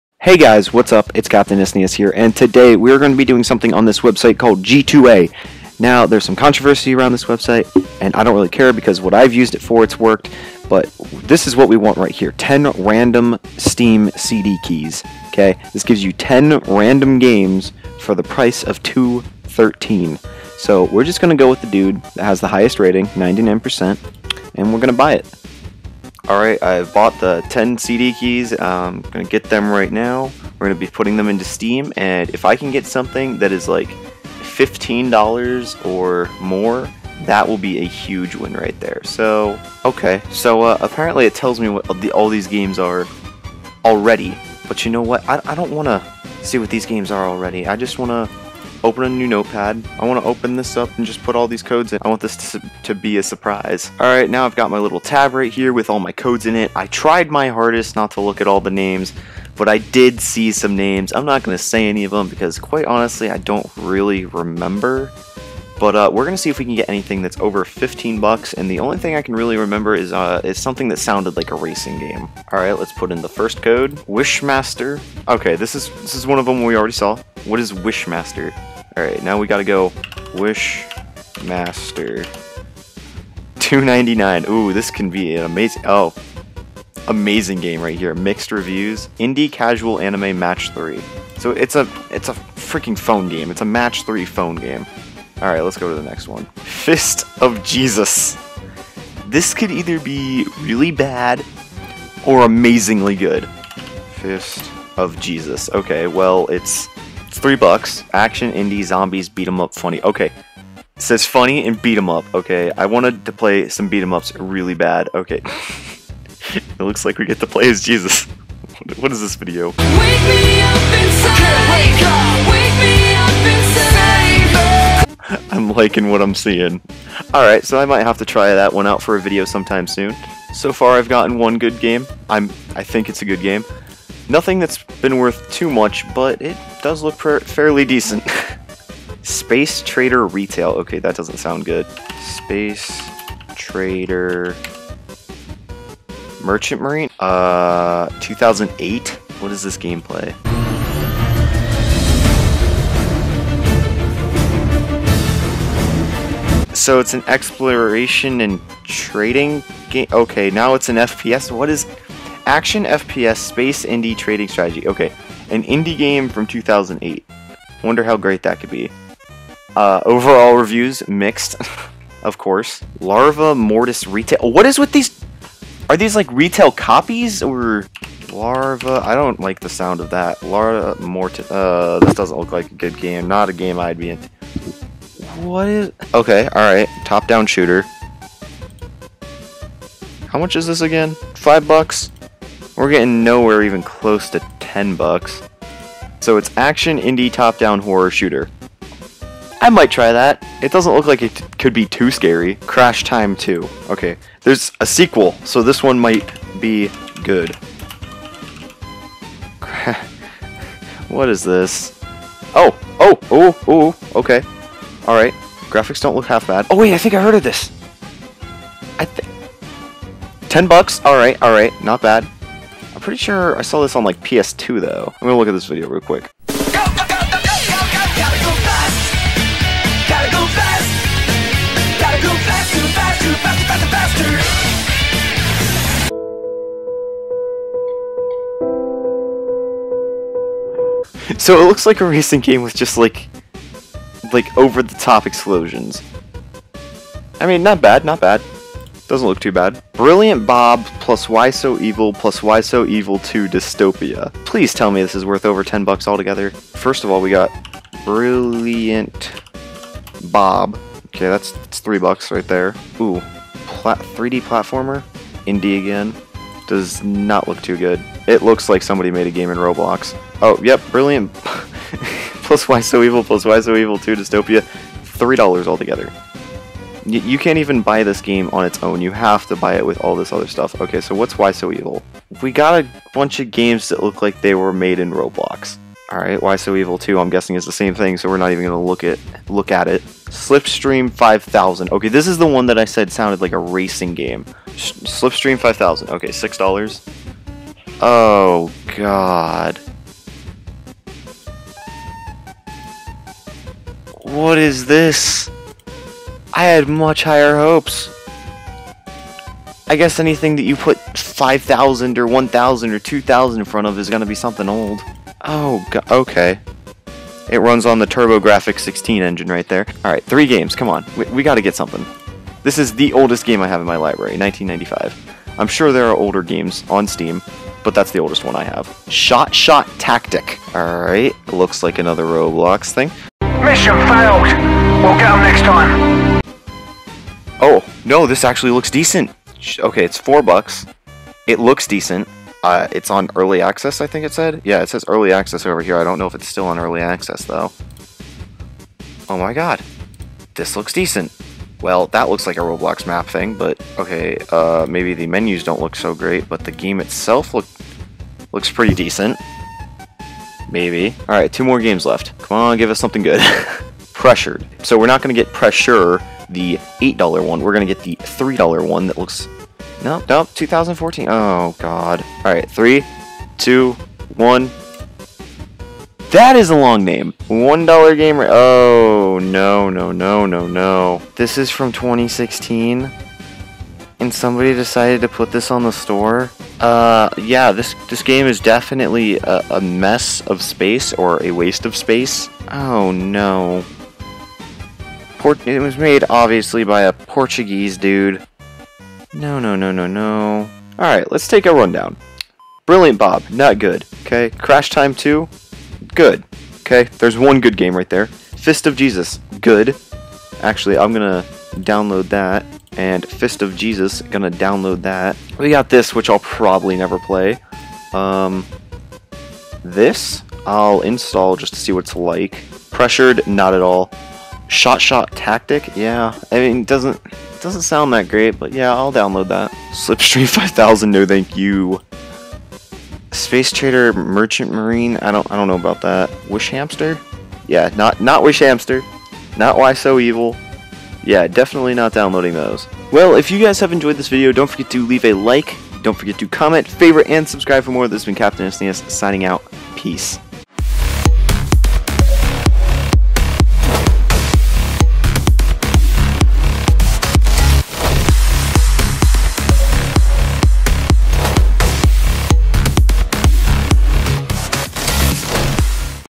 Getting 10 Random Games for $2.50
I thought for todays video i would get on g2a and get their 10 games for 2.50 offer and see what i get and see if its any good